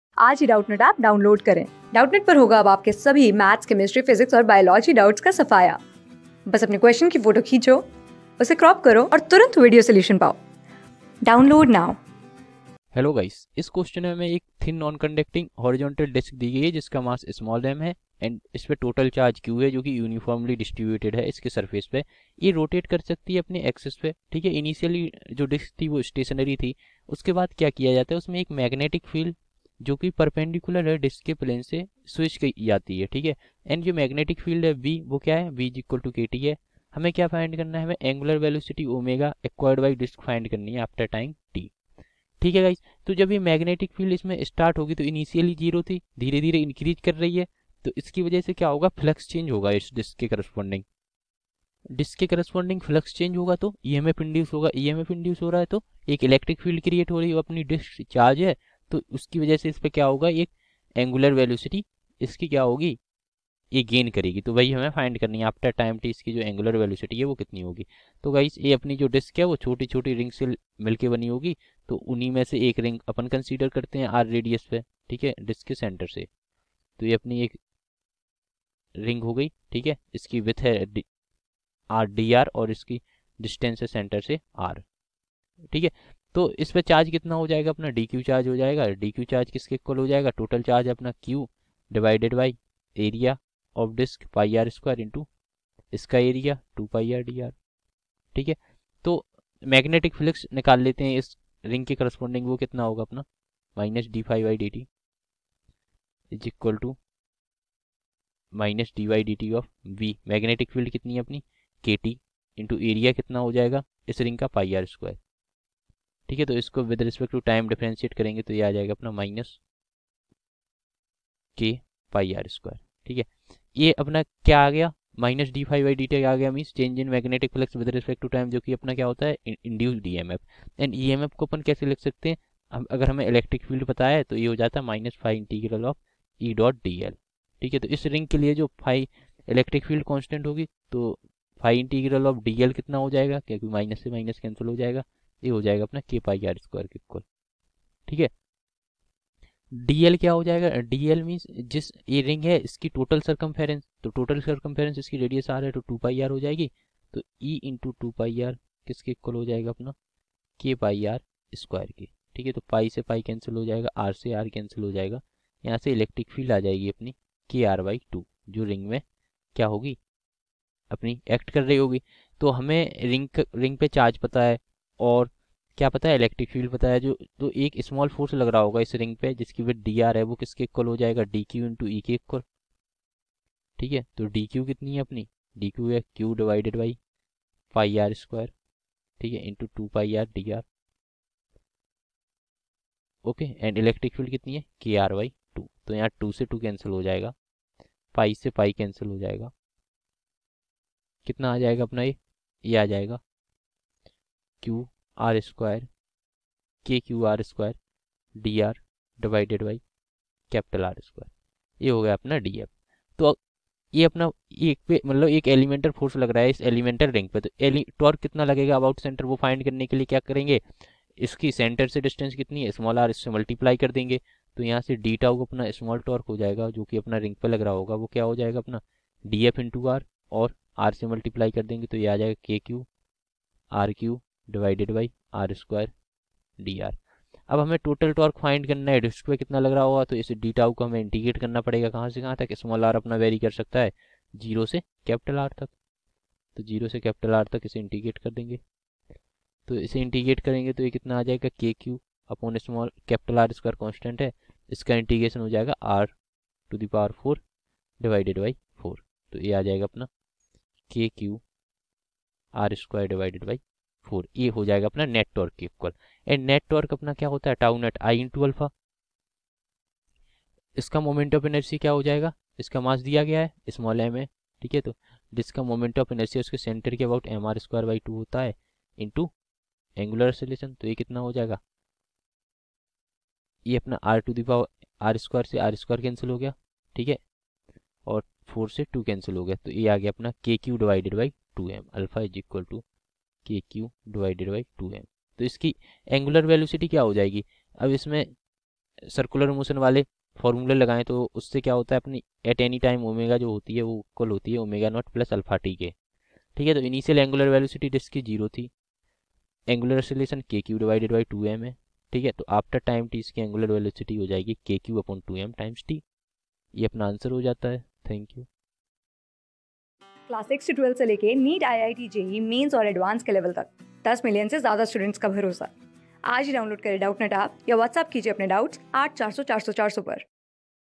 A thin non conducting horizontal disc of mass m having total charge q distributed uniformly over...
A thin non conducting horizontal disc of mass m having total charge q distributed uniformly over its surface, can rotate freely about its own axis. Initially when the disc is stationery a magnetic field B directed perpendicular to the plane is switched on at t=0. Find the angular velocity omega acquired by disc as a function of time, if B=kt, where t is time.
Class: 12
Subject: PHYSICS
Chapter: GMP ASSESMENT
Board:IIT JEE

👉 Have Any Query? Ask Us.